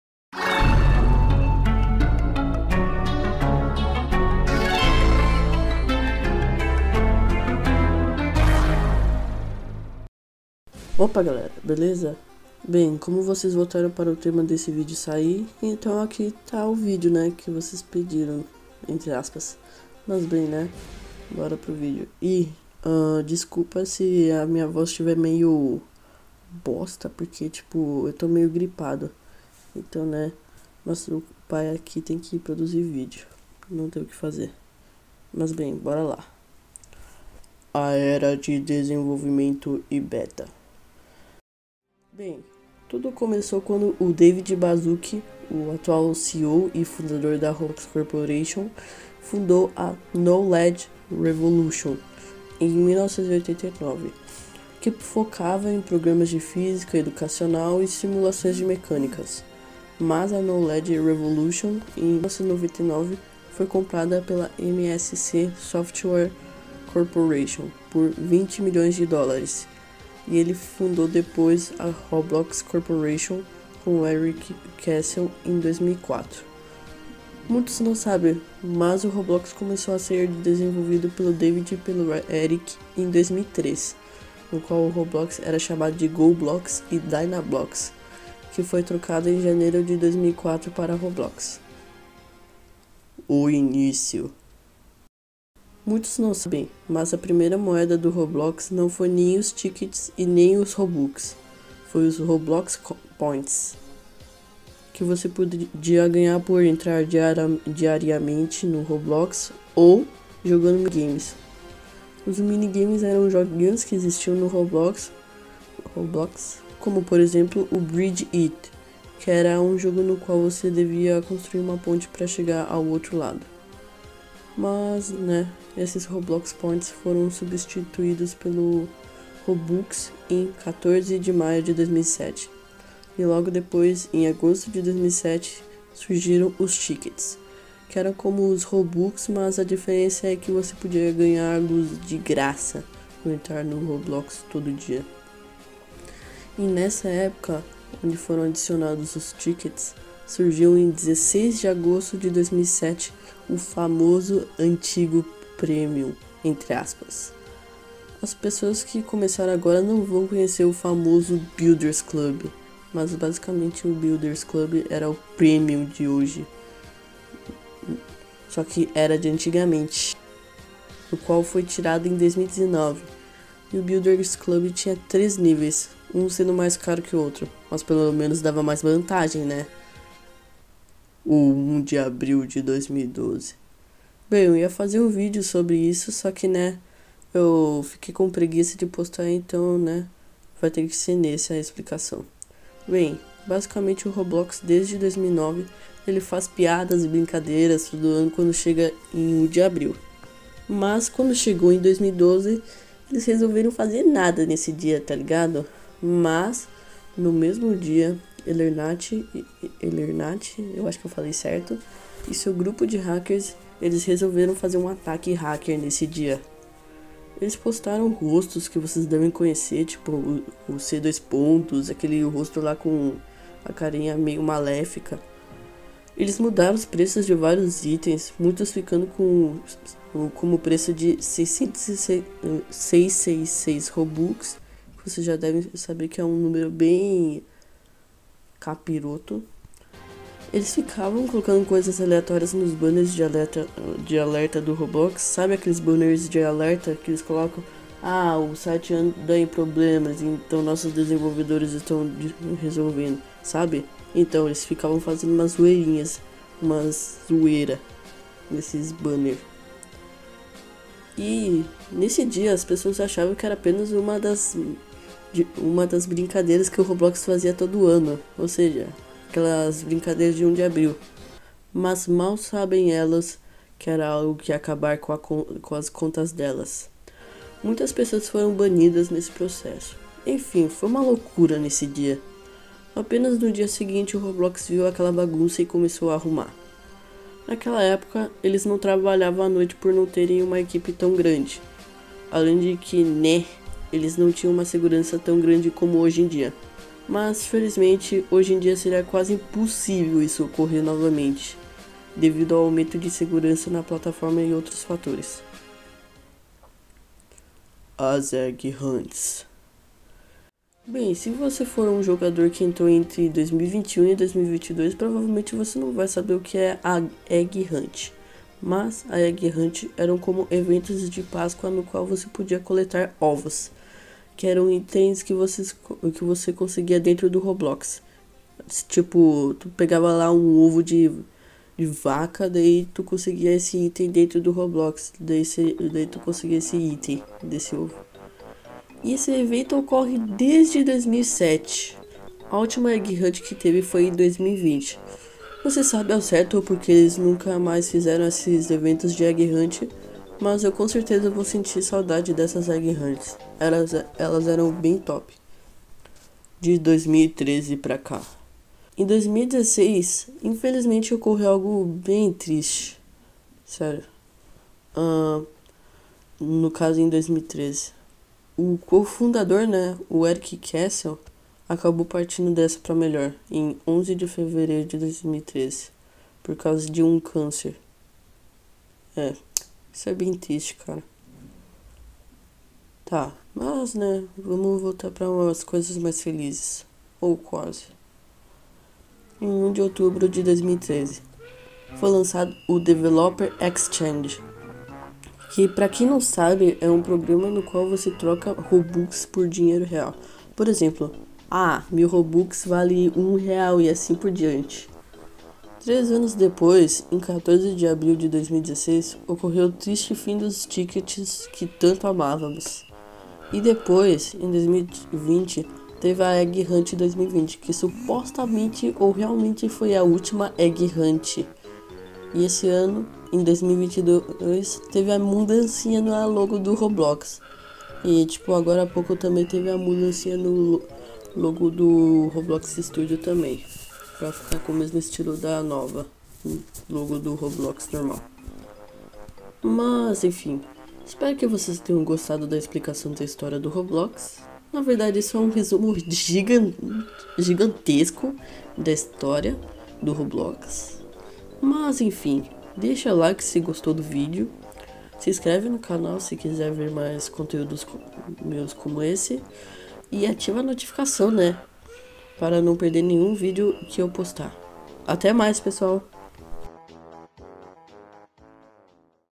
A HISTÓRIA do ROBLOX!
apenas um vídeo aleatório falando sobre a historia do Roblox.
Sumario -
00:00 - Tela de Título
00:10 - Começo
00:37 - A Era de Desenvolvimento e Beta
01:33 - O Início
03:00 - O 1 de Abril de 2012
05:07 -  As Egg Hunts
05:55 - De 2013 para cá
07:21 - Final